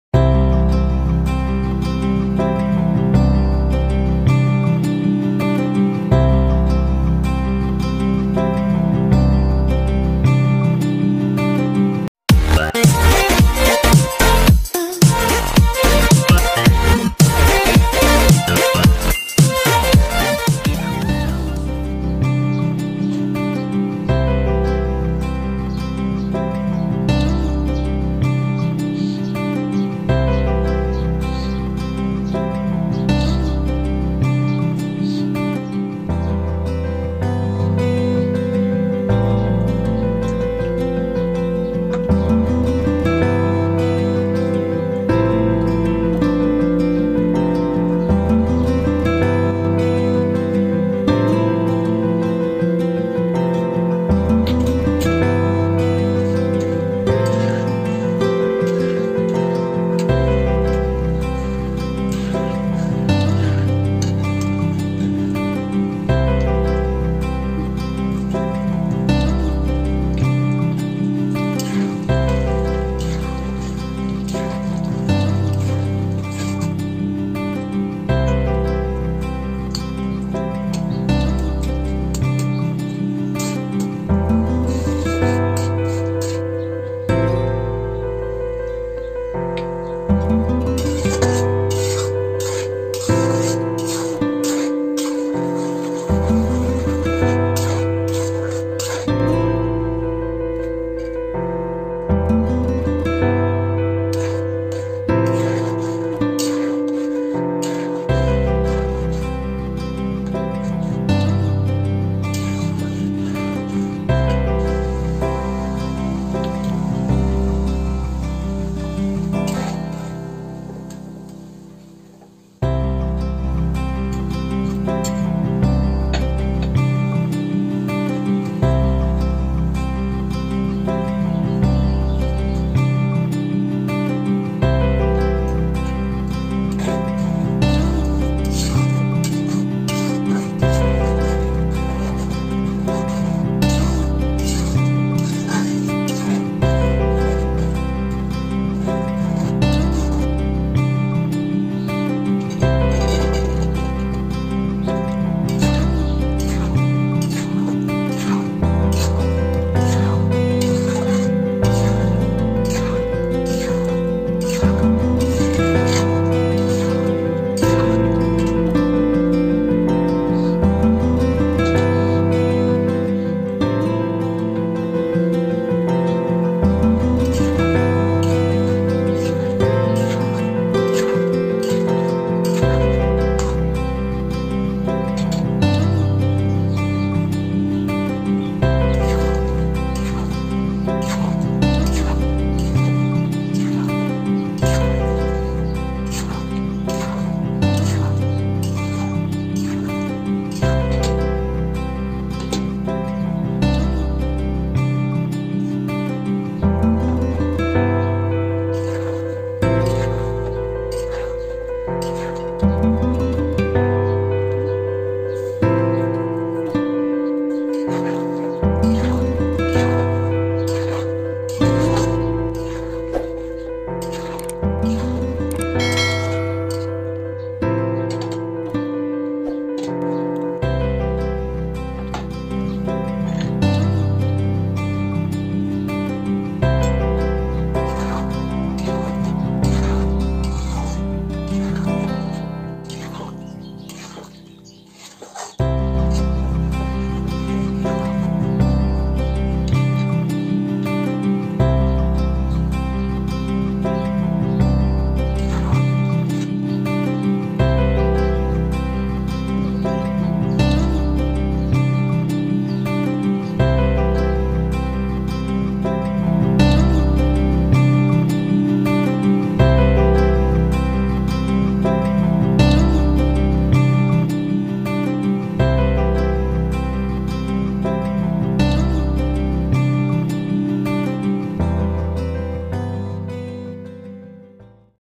তেলে ডোবা আস্ত জলপাইয়ের টক,ঝাল আচার। রোদে দেওয়ার ঝামেলা ছাড়া।(দীর্ঘ দিন সংরক্ষণ পদ্ধতিসহ)Olivesachar
তেলে ডোবা আস্ত জলপাইয়ের টক,ঝাল আচার। রোদে দেওয়ার ঝামেলা ছাড়া। (দীর্ঘ দিন সংরক্ষণ পদ্ধতি সহ)

How to make olives achar recipe. (Olive achar)

জলপাইয়ের আচার রেসিপি
protidiner rannabanna
potidiner rannabanna